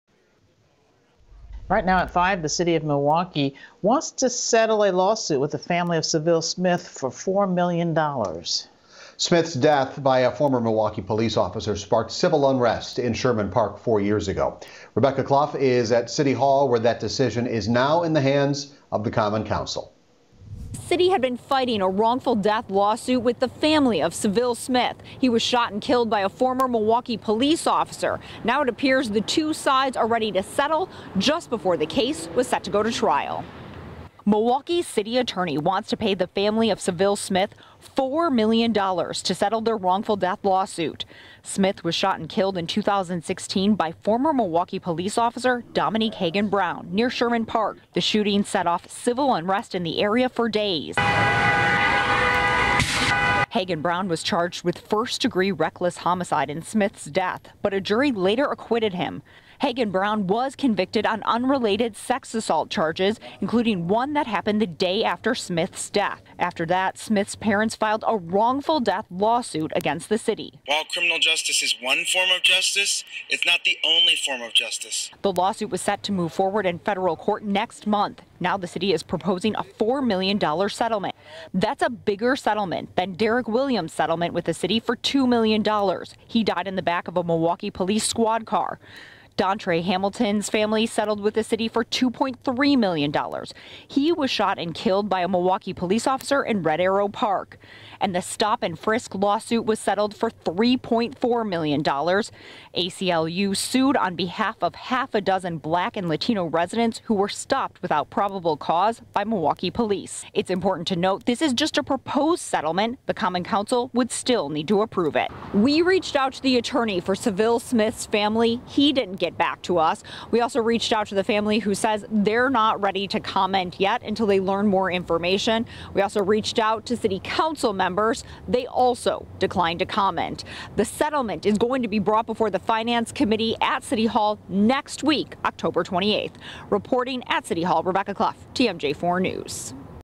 Milwaukee agrees to settle for $4 million with Sylville Smith's family
On Oct. 13, the city introduced a resolution to settle the wrongful death case Sylville Smith's family had brought against the city of Milwaukee for $4 million dollars.